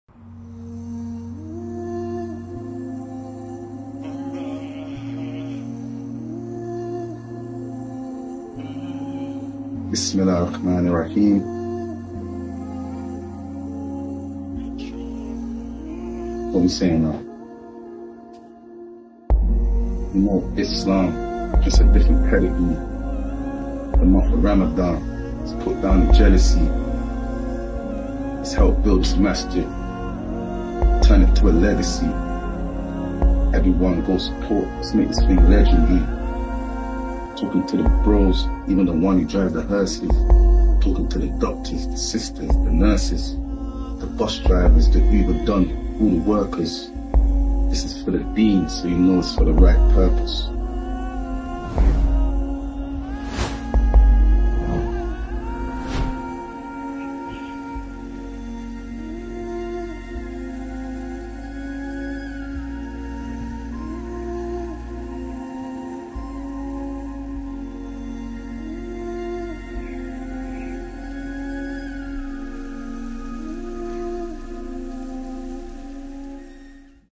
TRAILER 2 | Today's People, Tomorrow's Legacy | Message from RA (Real Artillery)
Today's People, Tomorrow's Legacy

Join us live on the 27th Night of Ramadan (Wednesday 27th Apri) at 6pm and help LIC lay down the foundation of a new mosque.

Why wait?